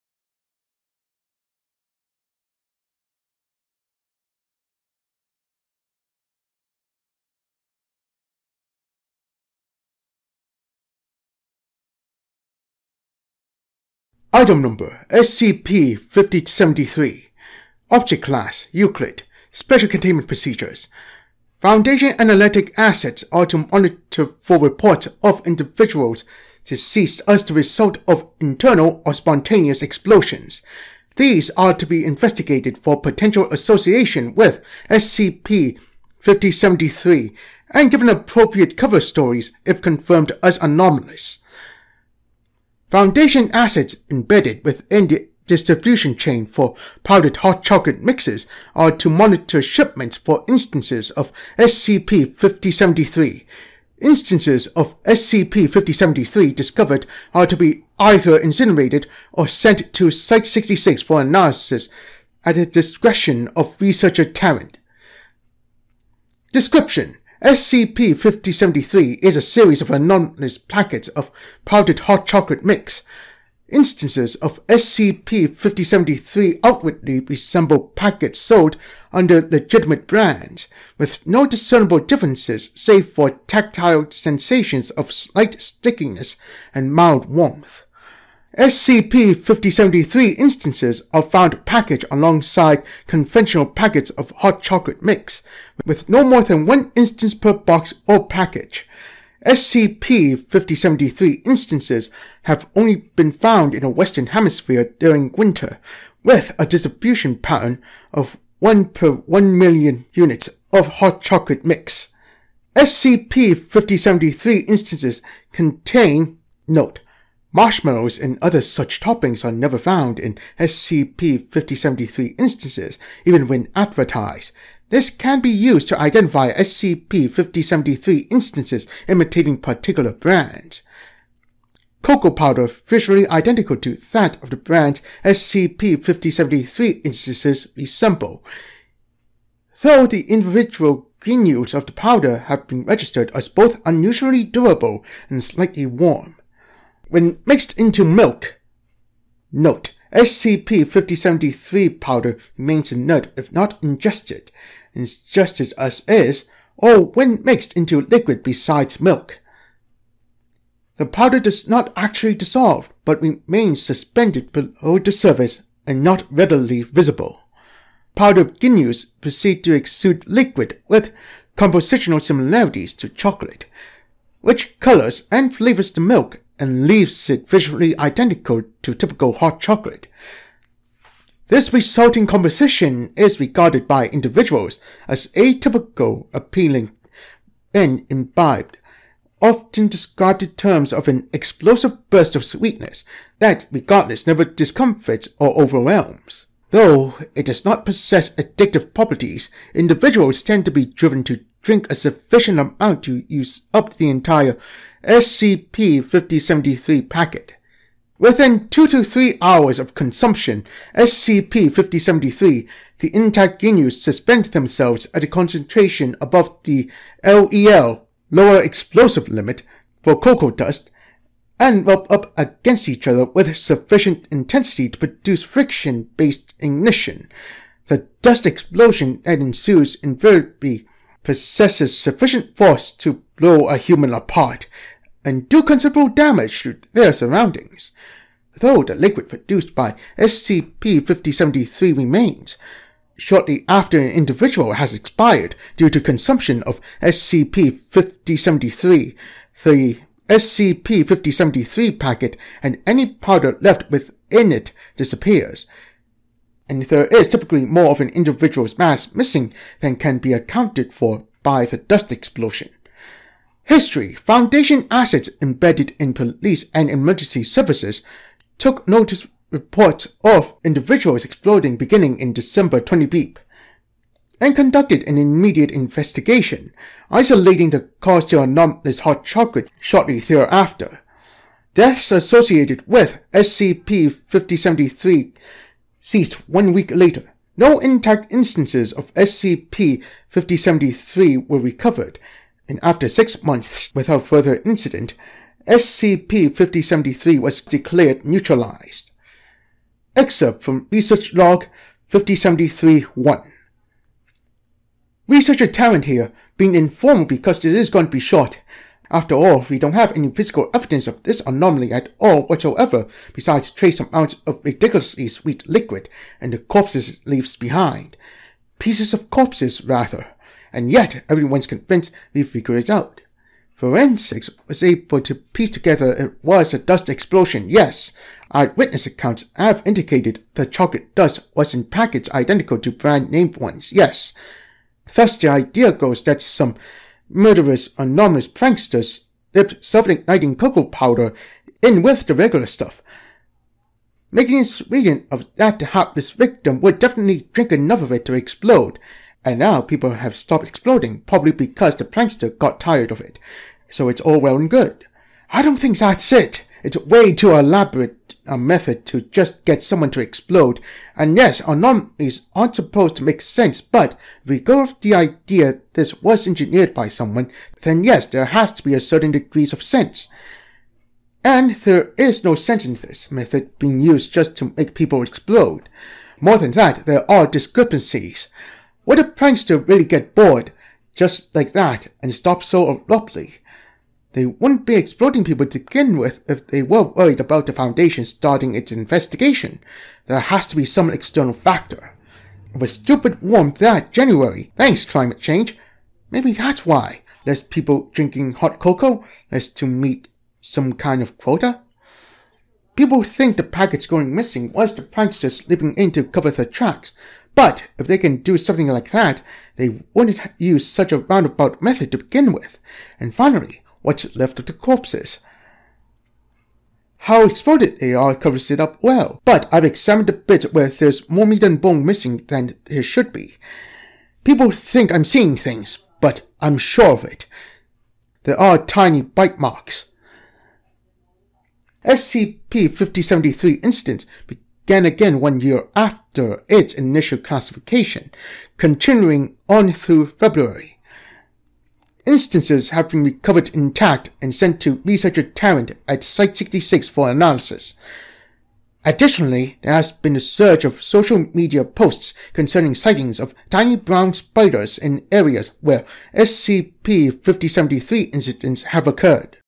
SCP 5073 Explosively Hot Chocolate | Delicious but dangerous SCP
This is one delicious drink if only not for the fact that it  can [redacted]